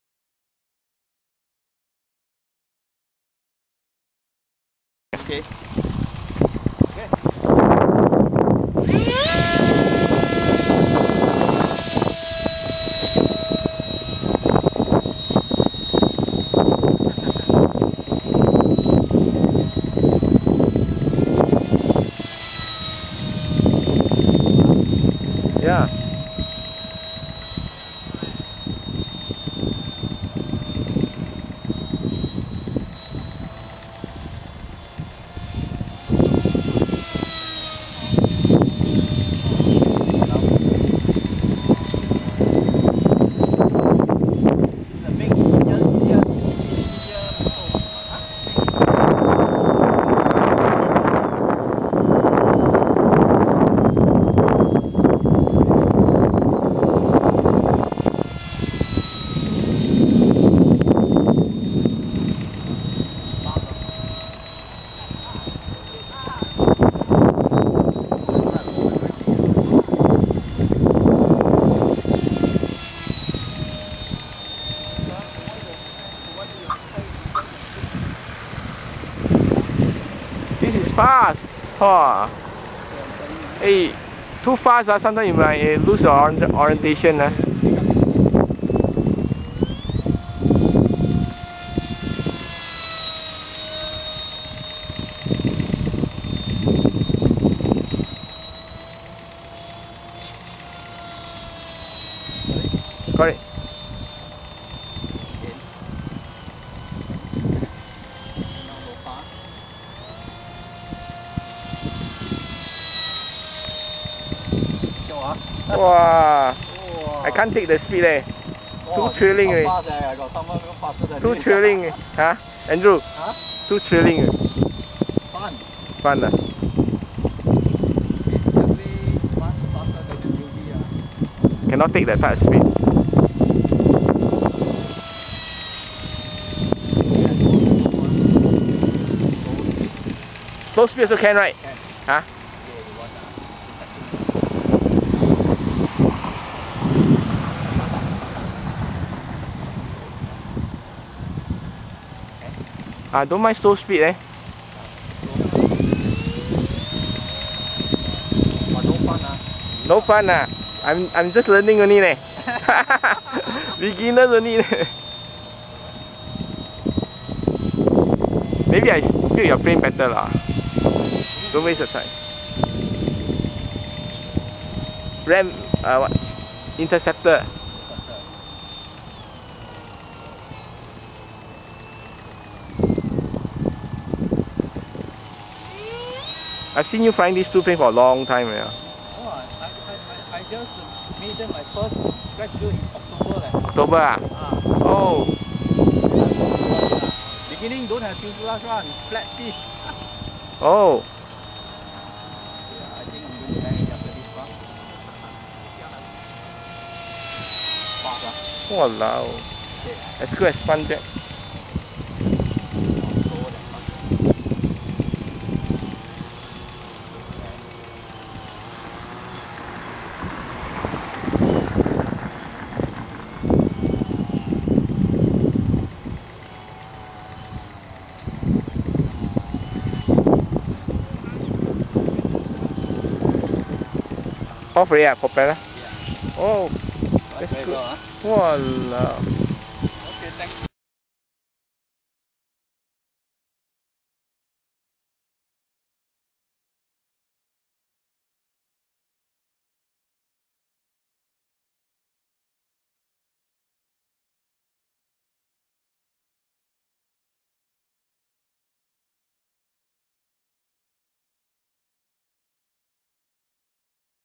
My micro EDF SPAD(corrugated plastic) interceptor
Radio control scratch built micro interceptor made of 2mm thick corrugated plastic material.
The wings are folded to get a tapered almost eliptical shape cord which makes the wing capable of high speed performance.
The aircraft is powered by 3cell 1750mAh 3S Lipo and 55mm 6 blade EDF and a 4.2KV edf out runner brushless motor. Very stable flying characteristics.
Best of all this plane is tough and almost crash proof.